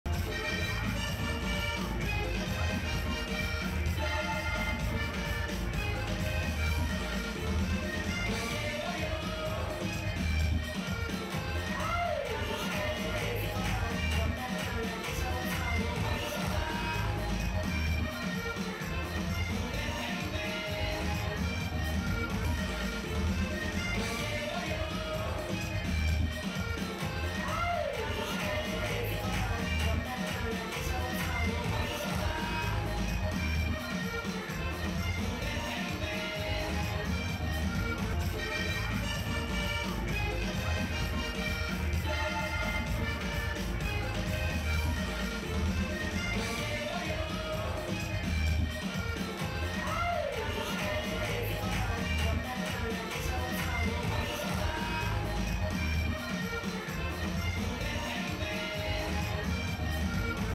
Sexy lap dance
She's a keeper! These beautiful, precious moments will forever be ingrained in those young birds' memories! Good lord the kids are having fun these days aren't they? No hate just love and dance baby DANCE! Latino music! in da club!